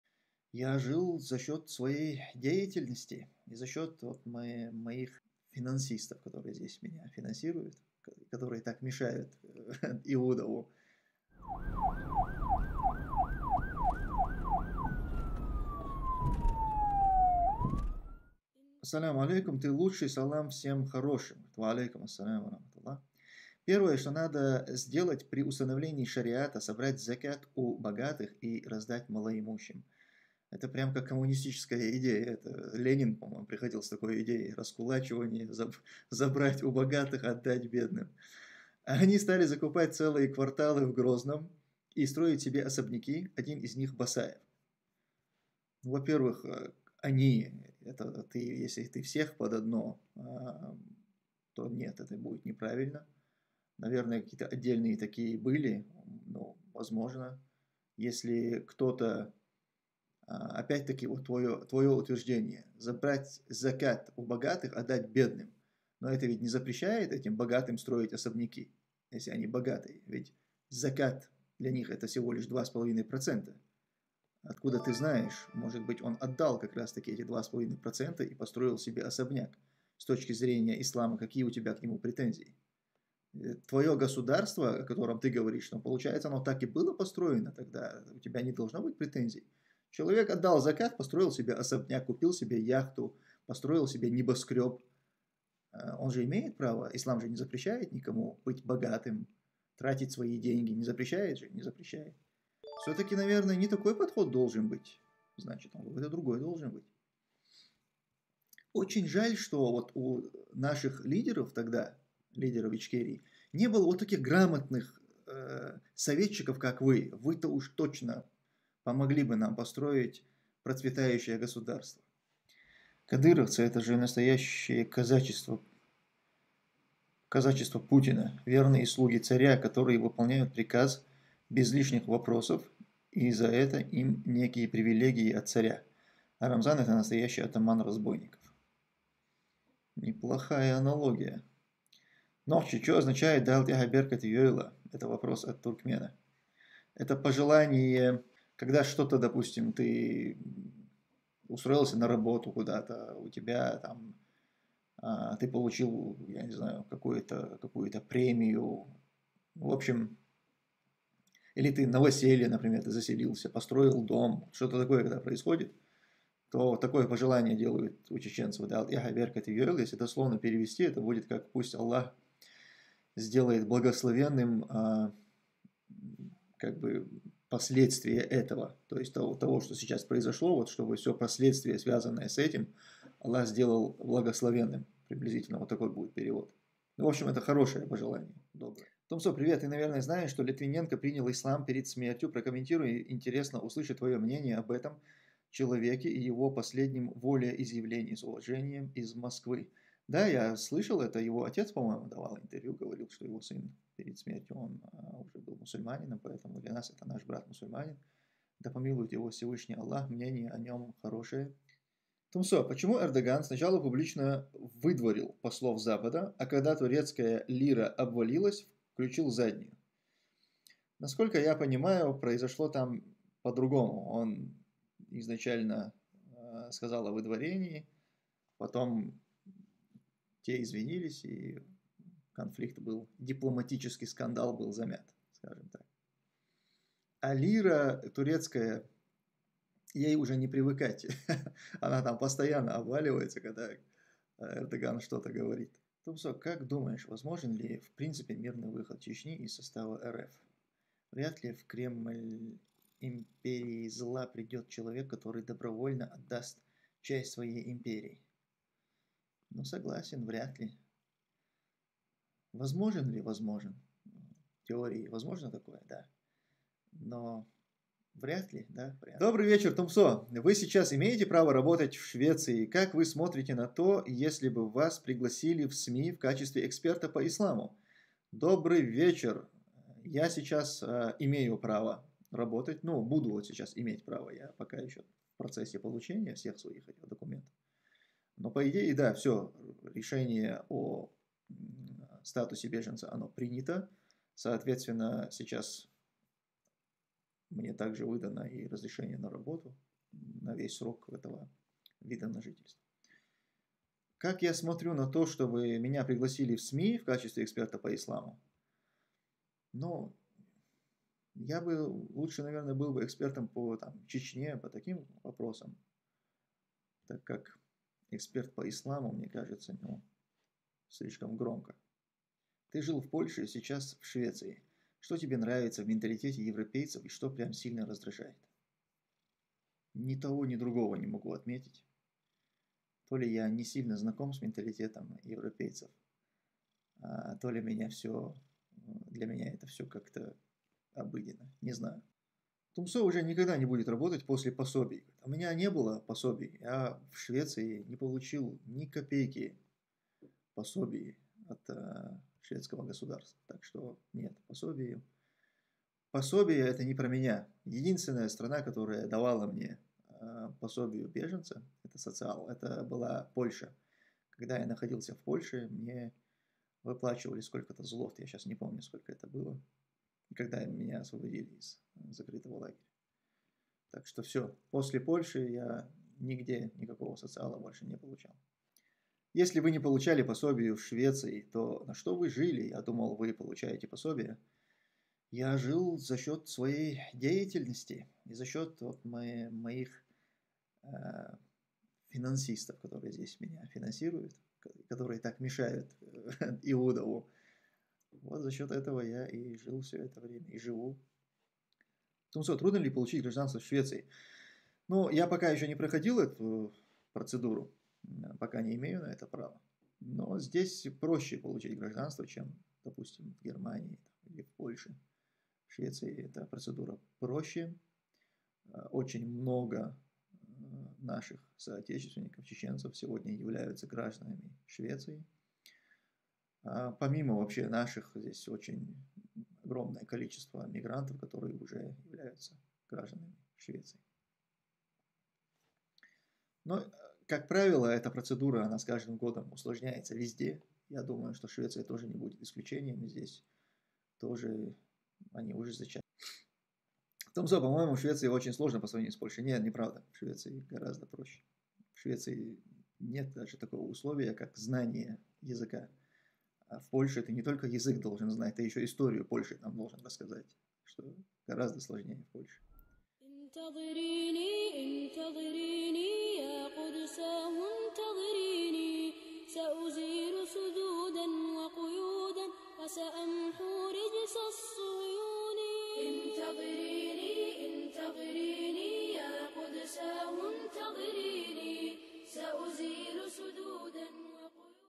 Я ЖИВУ ЗА СЧЕТ СВОЕЙ ДЕЯТЕЛЬНОСТИ И ФИНАНСИСТОВ КОТОРЫЕ ТАК МЕШАЮТ ИУДОВУ
ЖИЛ И ЖИВУ ЗА СЧЕТ ФИНАНСИСТОВ, КОТОРЫЕ ТАК МЕШАЮТ ИУДОВУ
ПРО ЖИЗНЬ В ЕВРОПЕ, ЛЮДЕЙ, ПОСОБИЯ, РАБОТУ И ГРАЖДАНСТВО
ЛИТВИНЕНКО ПРИНЯЛ ИСЛАМ